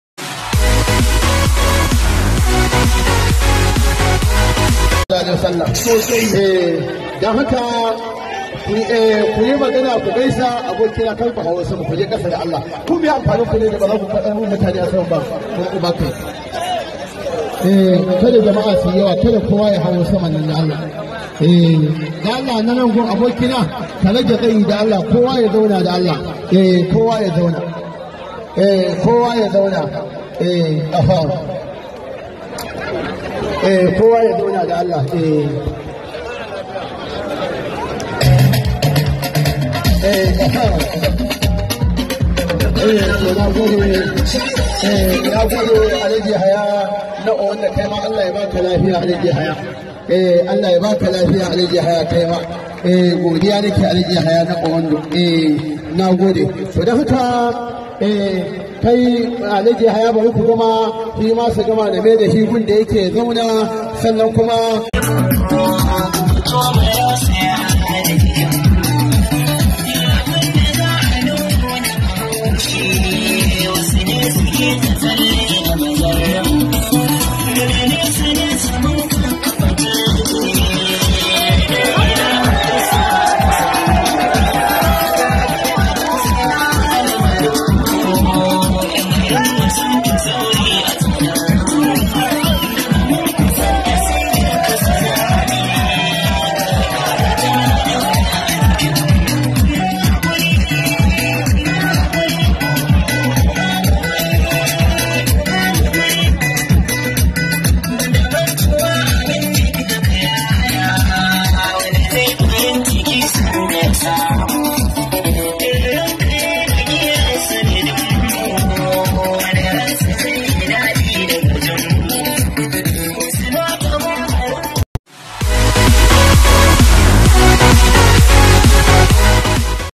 Sabon Taron (Nura m Inuwa) a kasar Niger. Kar ku manta Ku danna (subscribe) k
Shararen mawaki Hausa  Nura m inuwa ya gana da massoyan shi a kasar Niger